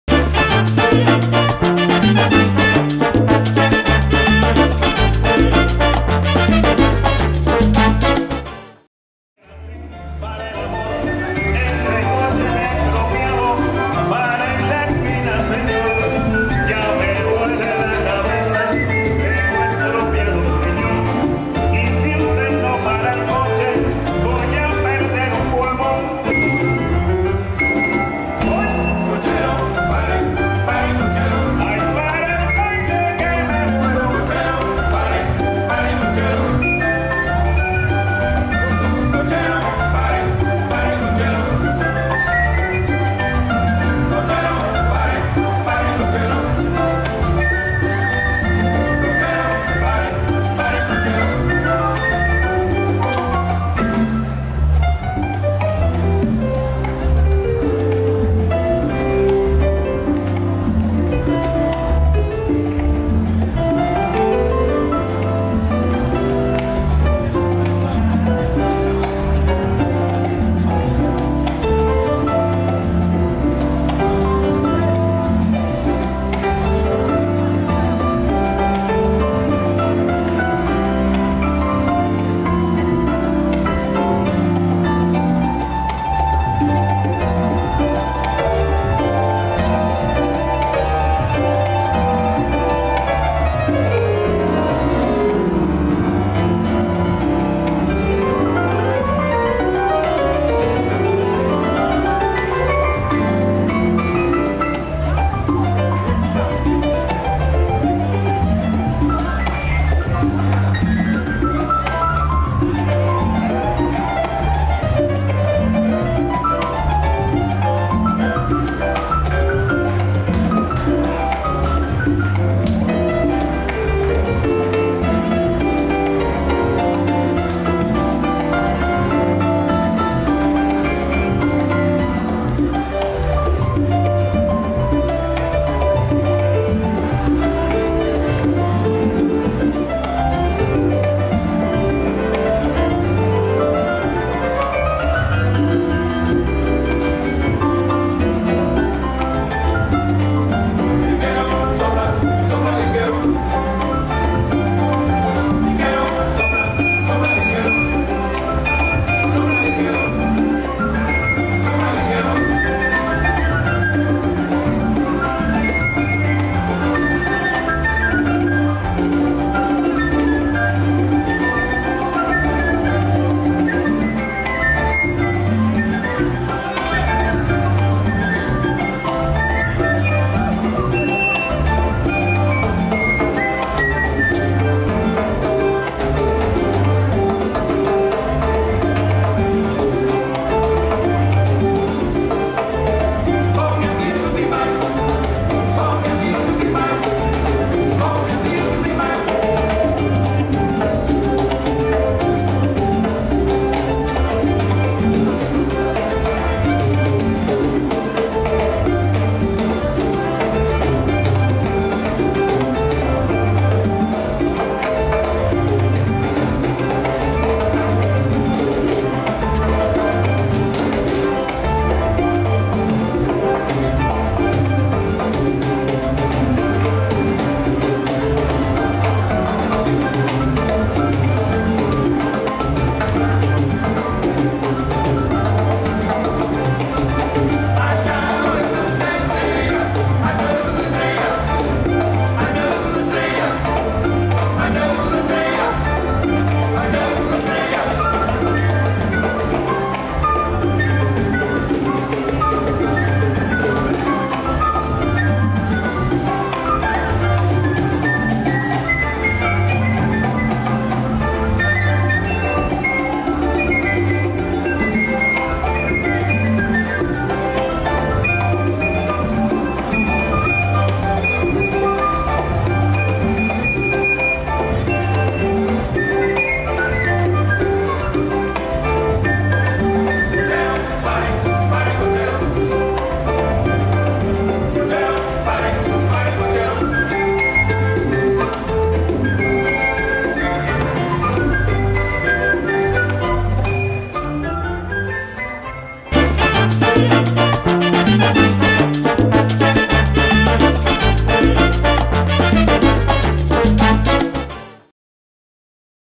Roland Stettler and Ebru Dargan Salsa Dancing at El Sol Warsaw Salsa Festival 2019, Sat 09.11.2019
Roland Stettler and Ebru Dargan Salsa Dancing at El Sol Warsaw Salsa Festival 2019, Saturday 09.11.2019